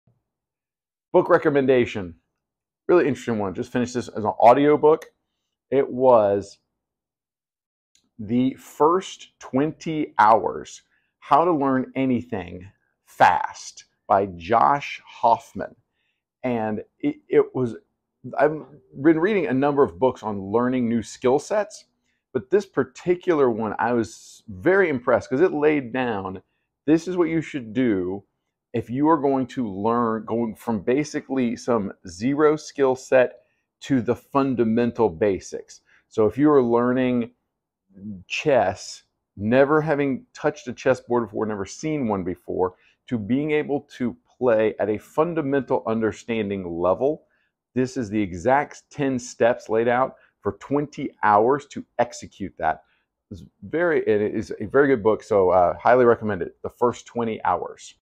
Book recommendation! The first 20 hours: how to learn anything fast by Josh Hoffman.
Hey everyone! 🤩 I just read a super helpful and interesting book, The First 20 Hours by Josh Hoffman, and it's amazing! 🤩 If you're looking for an effective way to learn almost anything quickly, this book is for you. 📖 It's filled with great advice and methodologies to help you maximize your time. 🤗 Check it out!